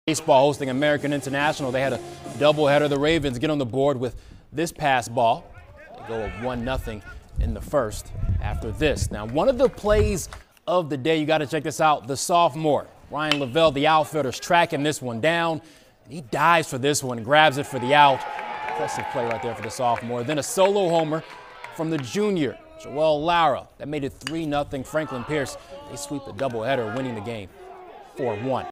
Franklin Pierce baseball sweeps double header against American International
FPU won game 2 of the double header 4-1.